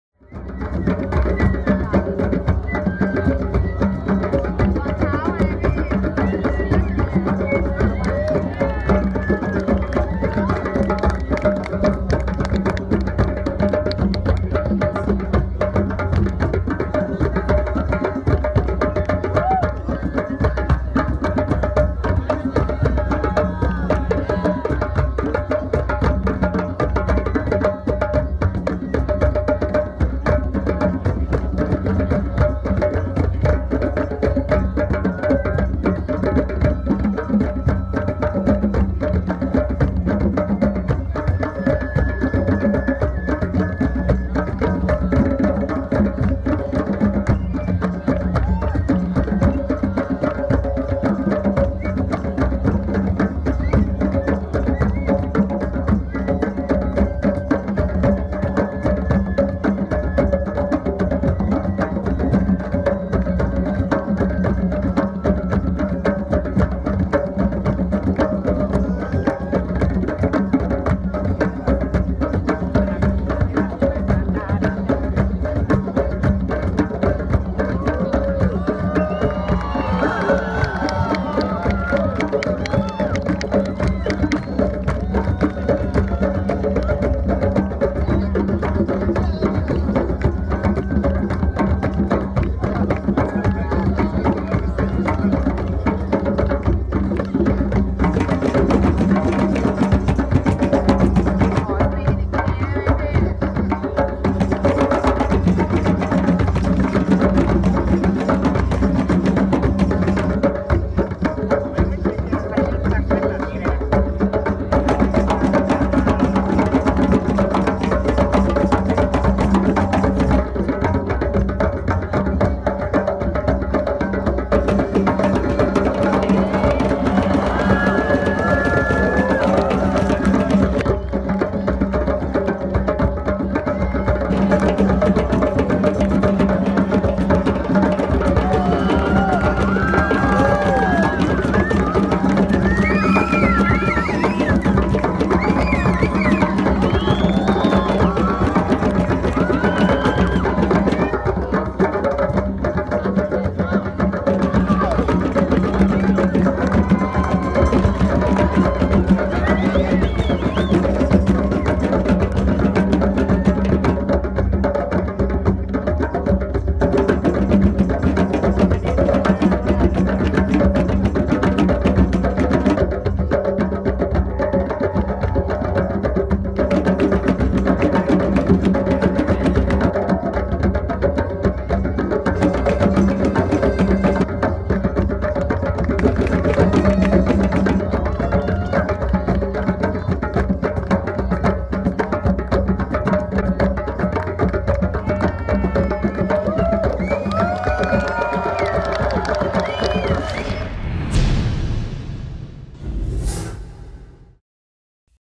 Shambala 2016 Thailand - Fire Hula Hoop Performance
Shambala in your heart is a hippie retreat festival type event. every night different acts perform and for a few nights we are lucky enough to see some amazing fire performers. One in particular was my friend Laura whom Id met and spun with in Chiang Mai. this is her performance at Shambala 2016 in Chiang Dao, Thailand